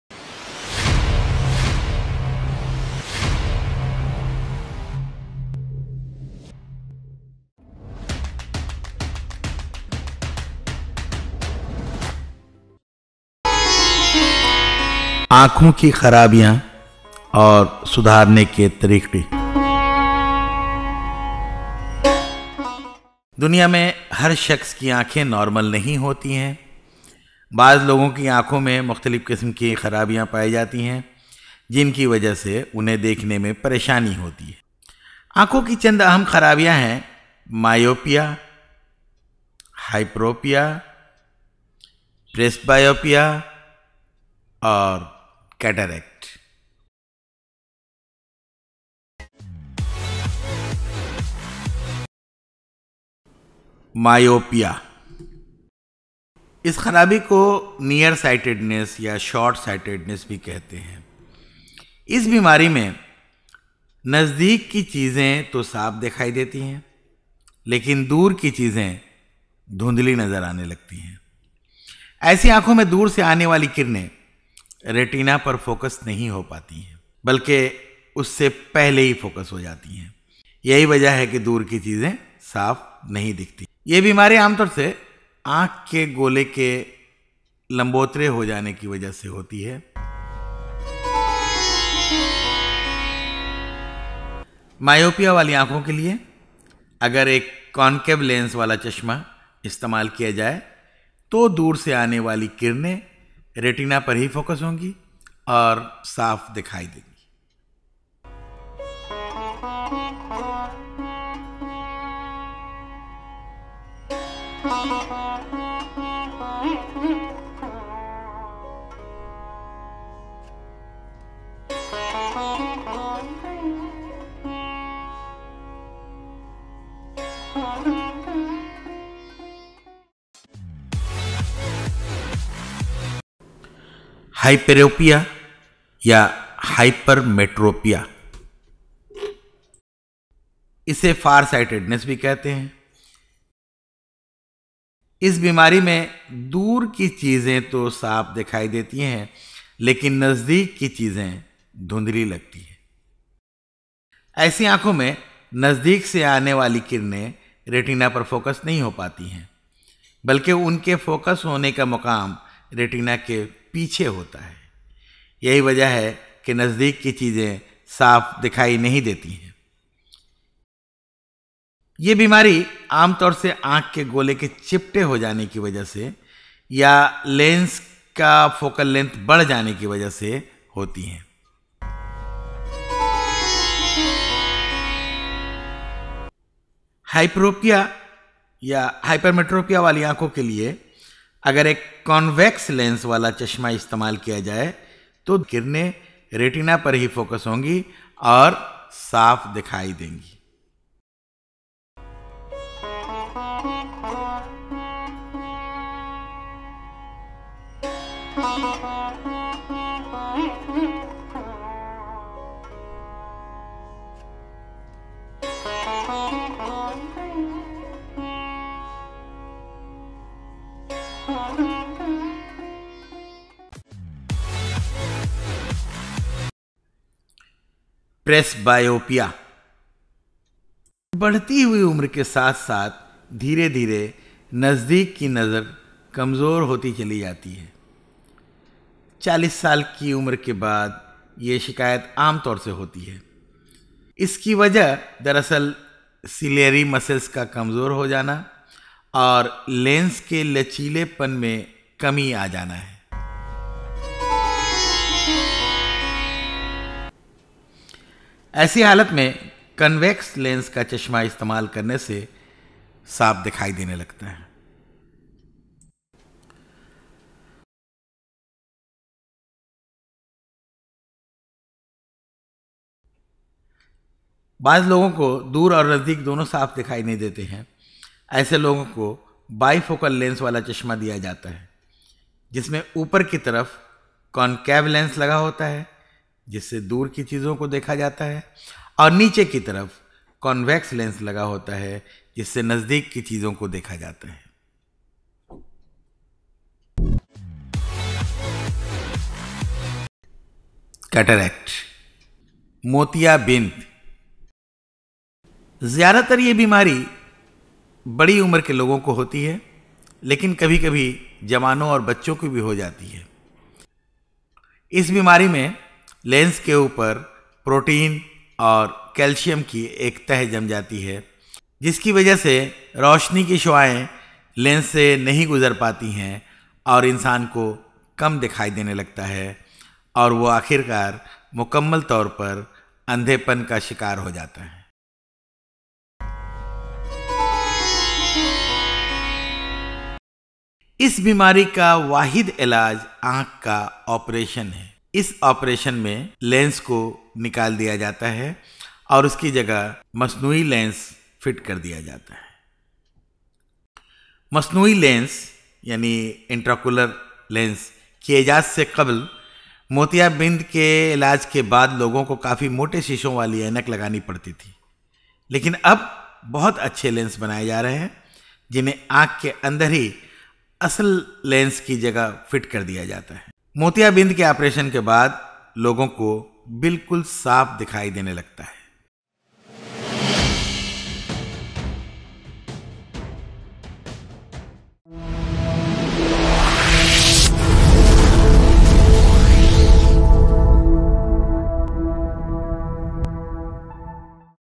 Defects of vision and corrective measures | آنکھوں کی خرابیاں | ऑंखों की ख़राबियाँ
An Urdu / Hindi video of Biology for the students of class 10, by Dr. Khurshid Eqbal about the defects of vision and corrective measures.